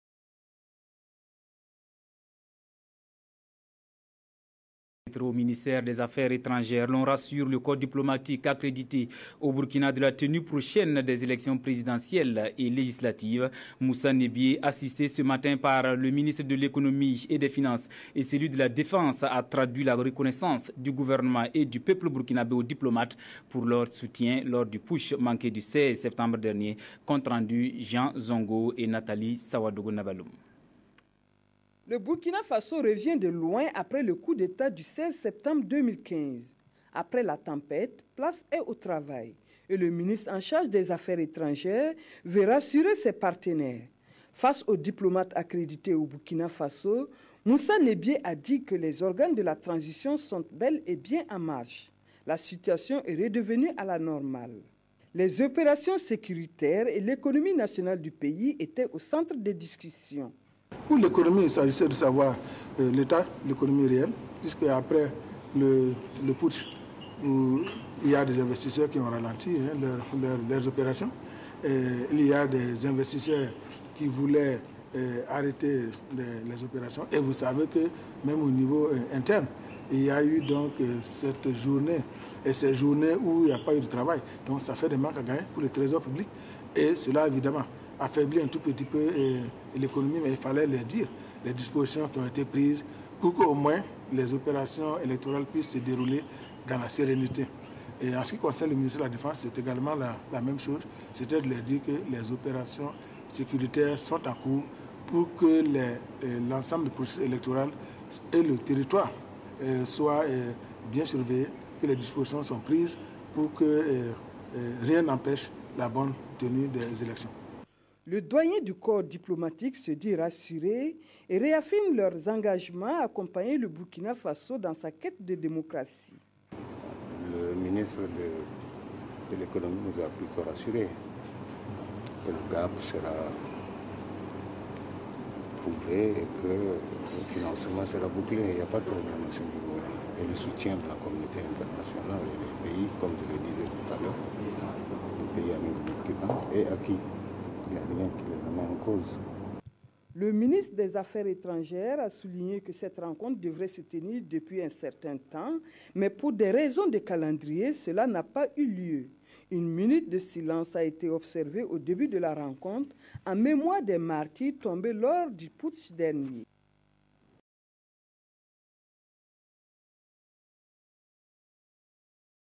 RTB/Politique-Le gouvernement rassure le corps diplomatique de la tenue prochaine des élections
Le gouvernement rassure le corps diplomatique de la tenue prochaine des élections présidentielles et législatives